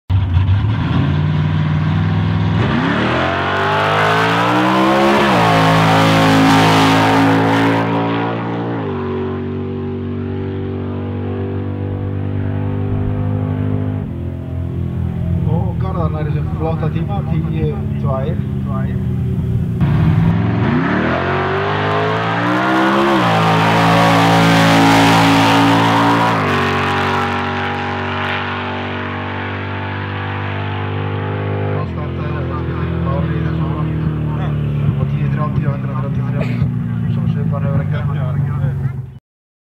1976 Plymouth Road Runner
1976 Plymouth Road Runner at the 3rd round of the Icelandic championship in drag racing, July 25 2015.